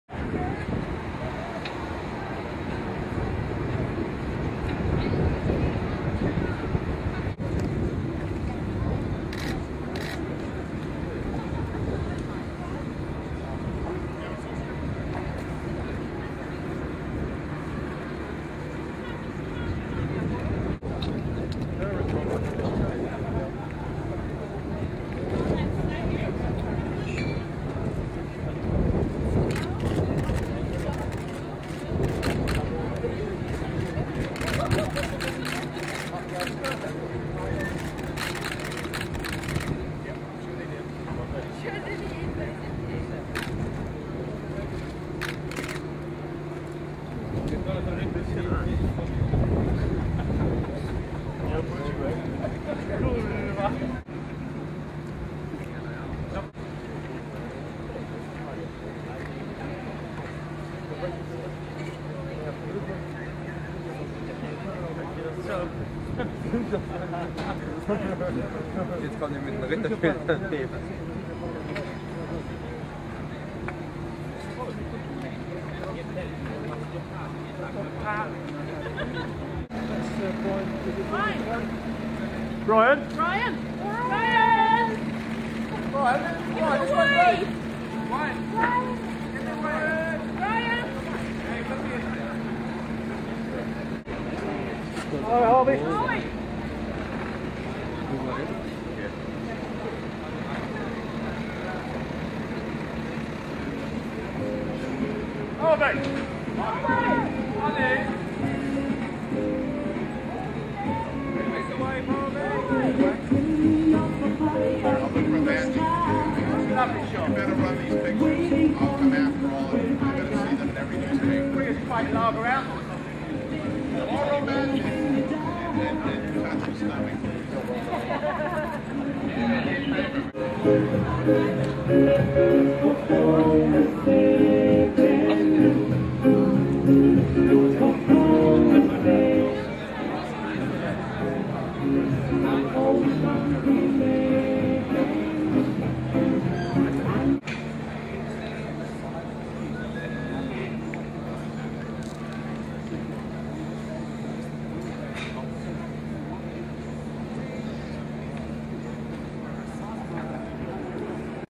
Colin Firth and other guests at "Before i go to sleep" party on a boat in Cannes
Colin Firth accompanied with wife as well as producer Harvey Weinstein and other actors attended a party on a boat for the film "Before i go to sleep"

Cannes, France, 17th May 2013